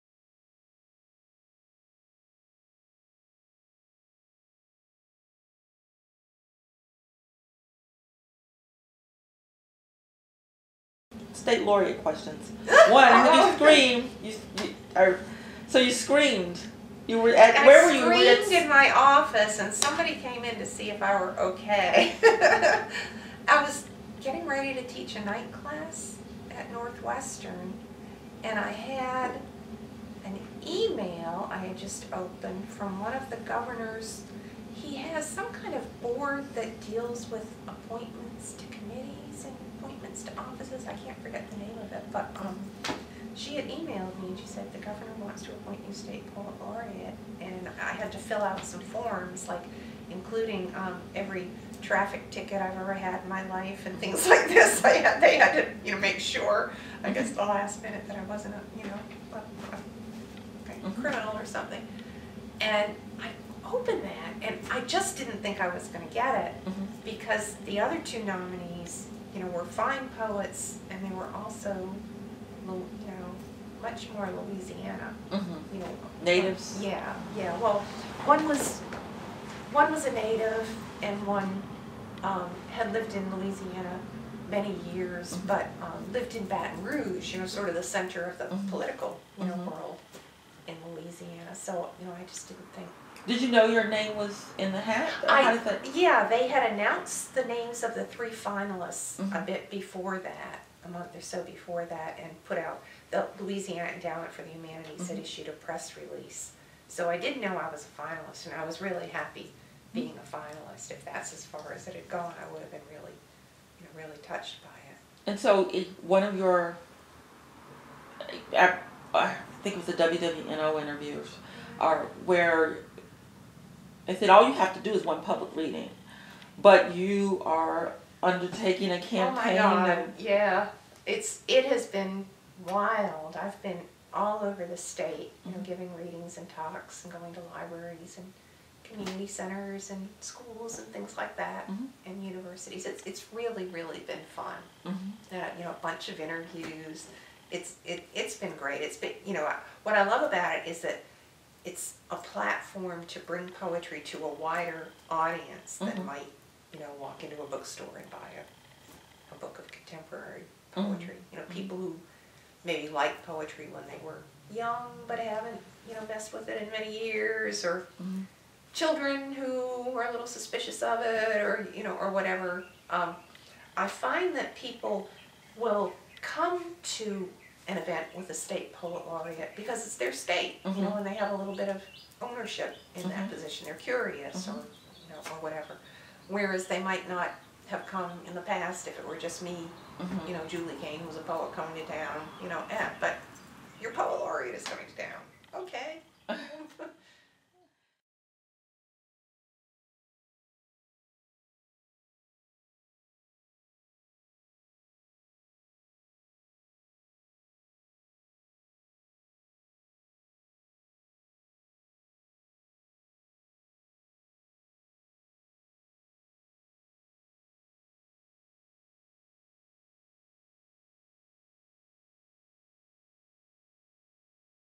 Julie Kane State Poet Laureate
Julie Kane recalls when she first learned that she was Louisiana's poet laureate for 2011 through 2013 and discusses her campaign to expose more people to poetry.